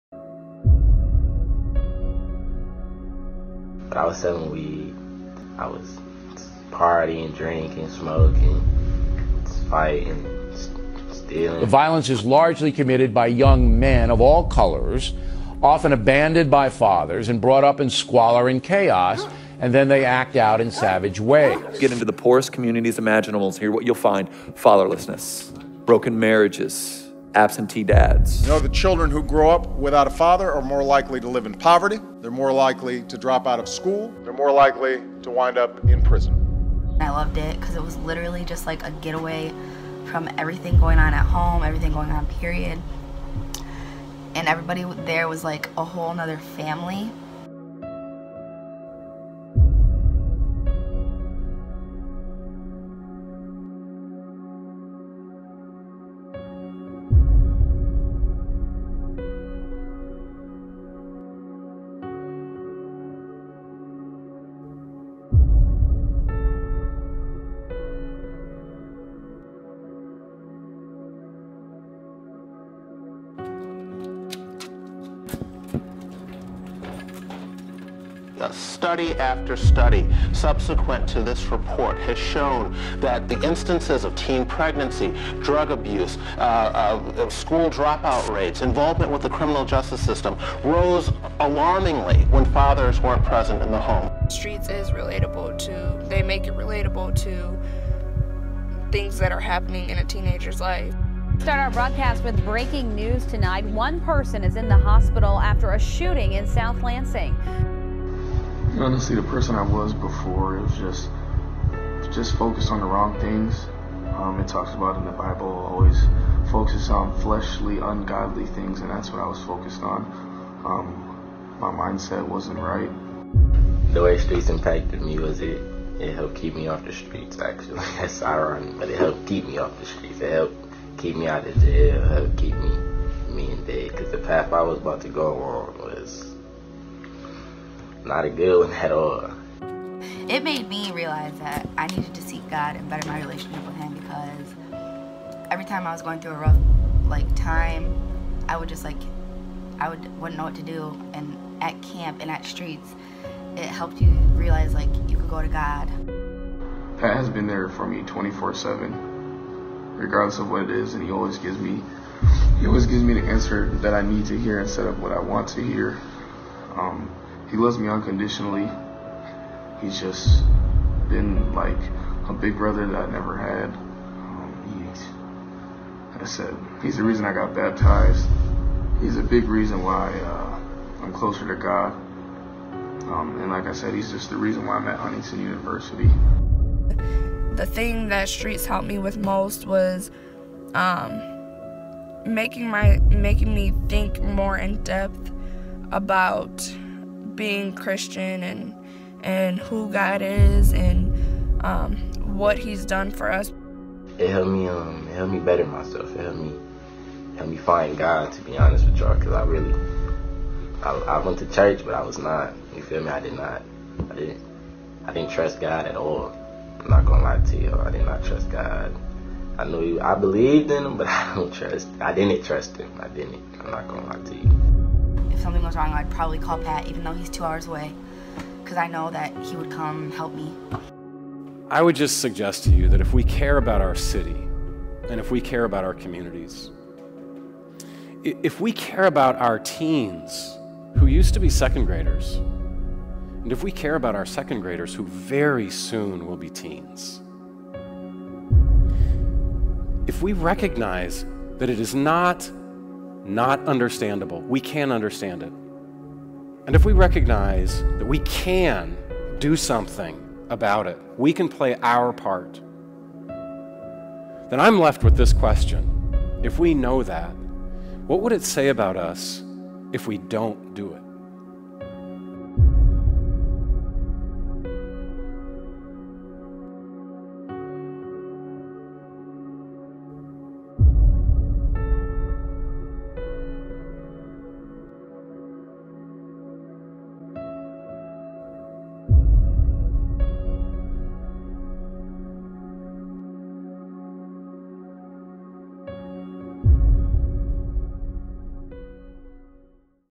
What Will Your Role Be?
Would you keep STREETS 517 Ministries in mind this year-end giving season?

"STREETS 517 Ministries strives to serve the urban and underserved youth of Lansing through educational, mentoring, recreational, outreach and other faith based programs"